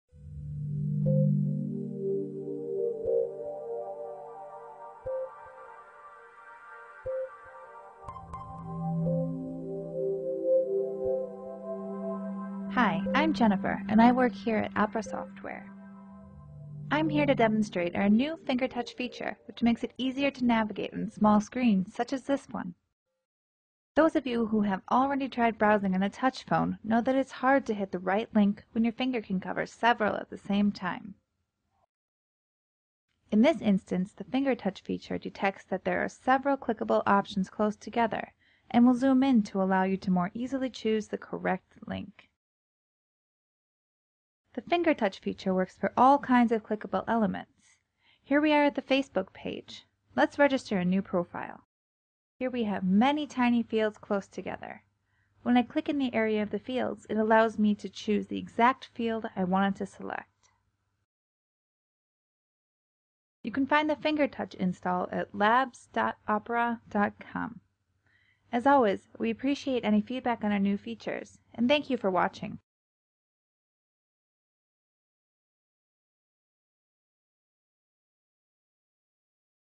Opera Mobile 9.5 Beta2 : FINGERTOUCH UIQ3 Sonyericsson
Nouveau logiciel du navigateur opera mobile version 9.5 beta 2 tactile . Plus de détails sur le site de my- opera communauty ; info de DOWNLOAD pour P1i ,W960 , G900/700